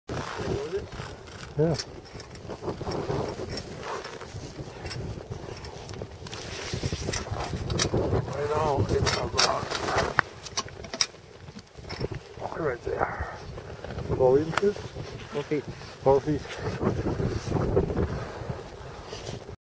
20140103 01 SYS4 Reraise Ruler @ South Pole
A ruler was set next to the pit to show how deep the pit was this year.